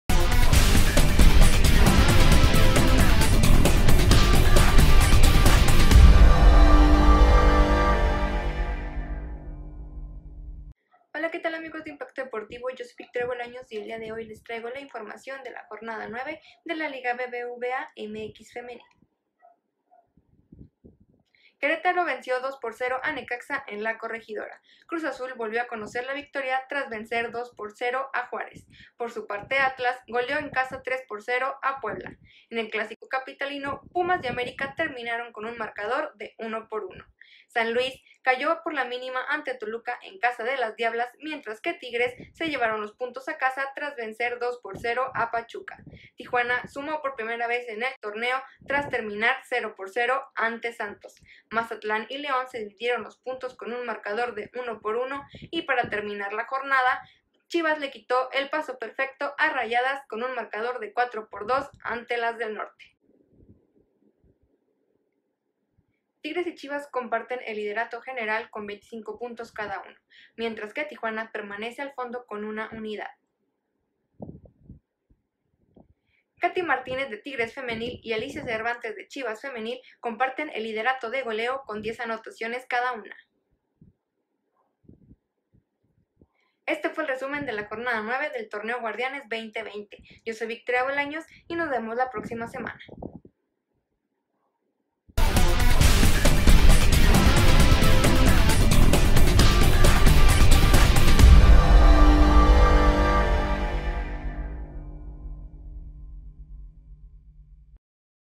Se disputó la jornada número 9 de la ligamxfemenil
Resumen de lo que dejó la fecha del fútbol femenino en México, Victoria Bolaños nos cuenta.